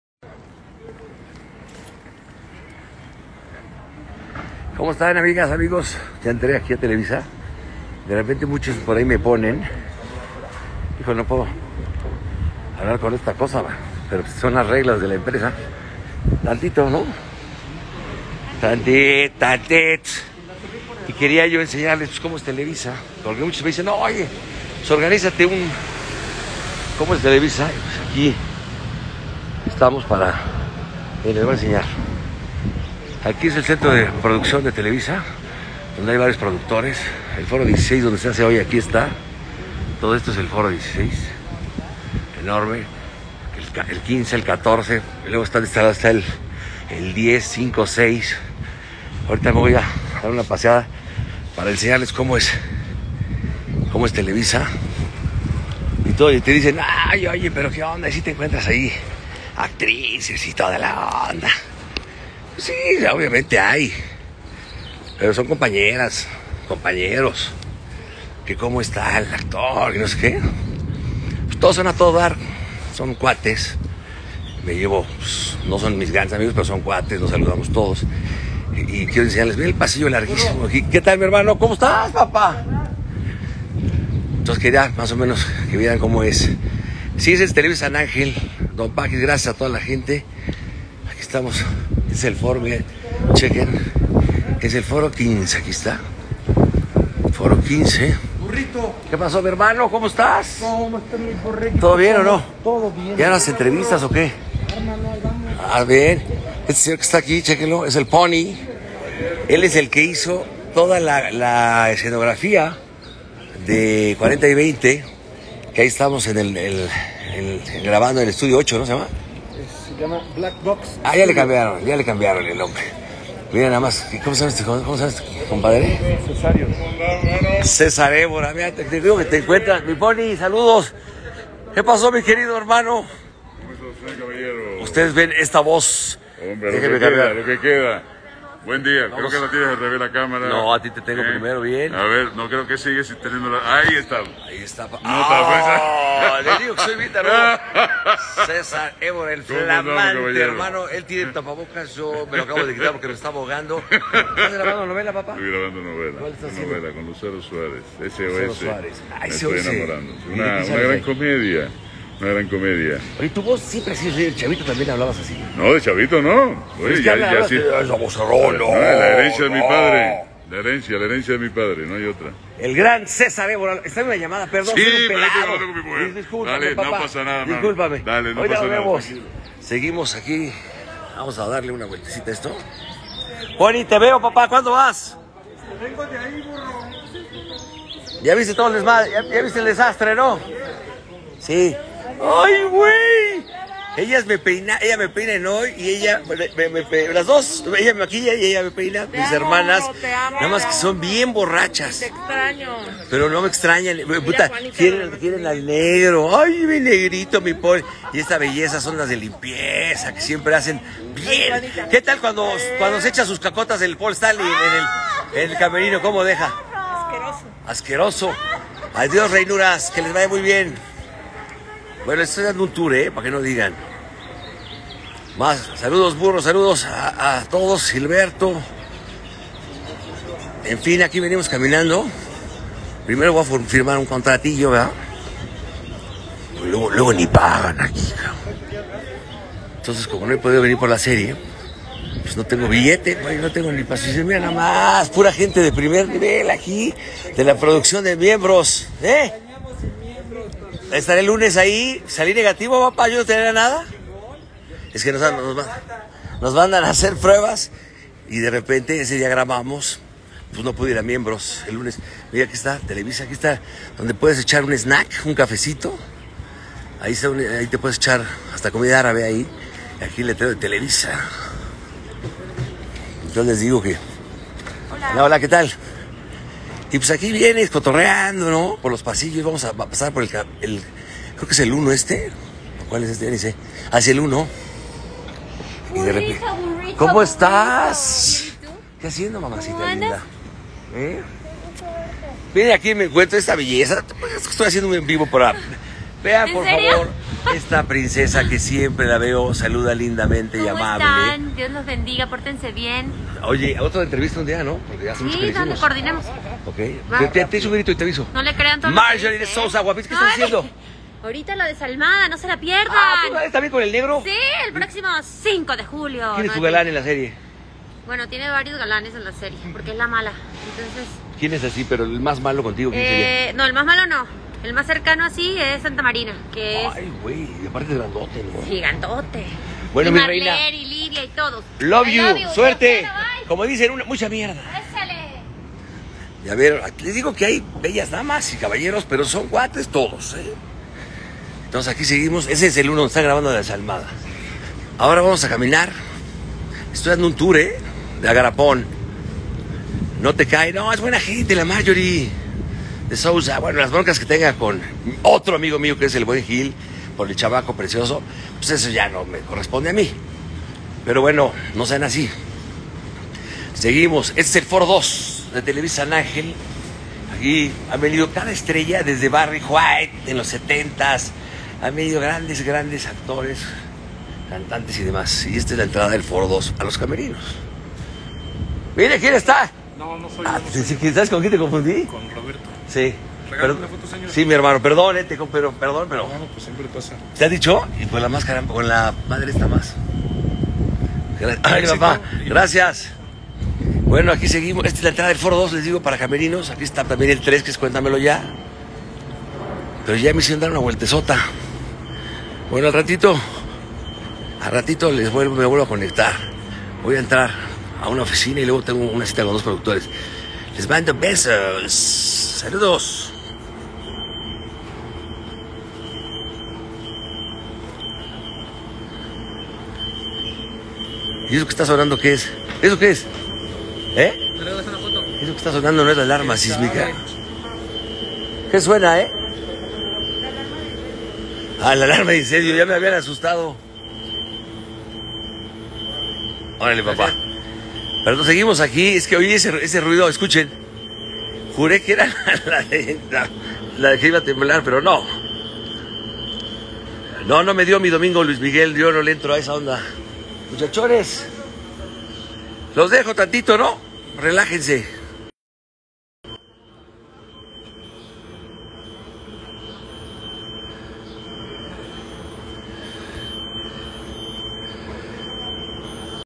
UN DIA EN TELEVISA | UN DIA CONOCIENDO LOS PASILLOS DE TELEVISA
un dia visitando y conociendo televisa por dentro.